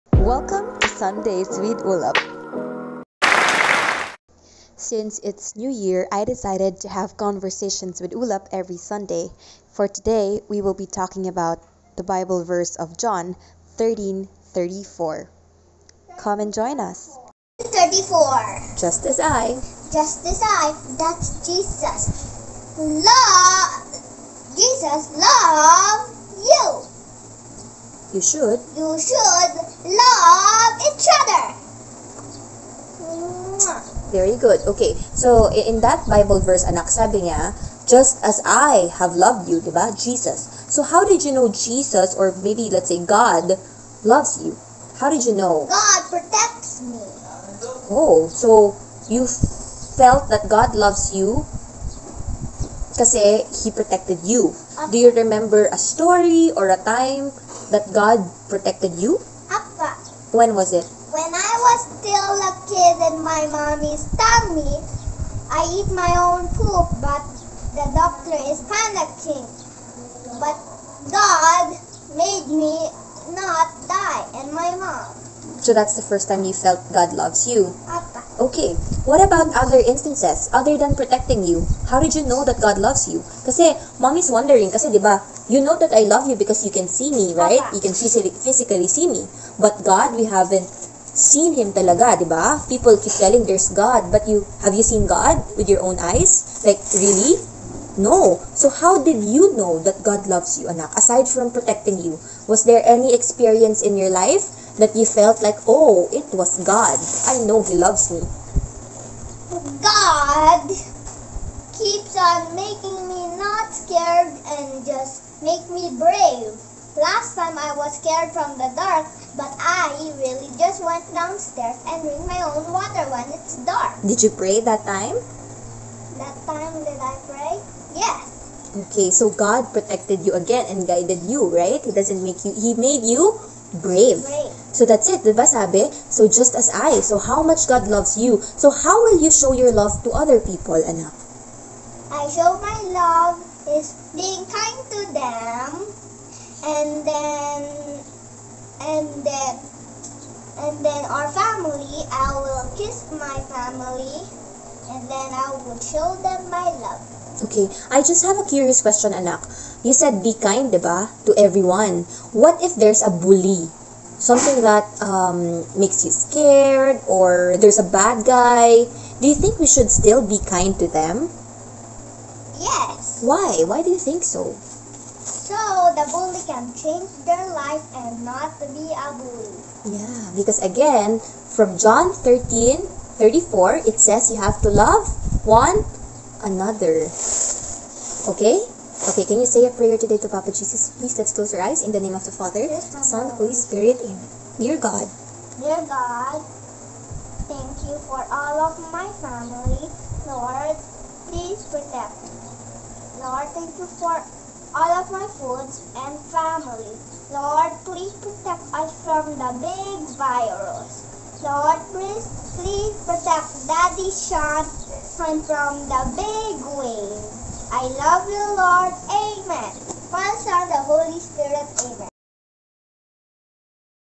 VLOG #1 SUNDAYS WITH ULAP " JOHN 13:34"
note: Not good in video editing. just wanted to share my conversations with my son after learning a bible verse every  Sunday :) God bless;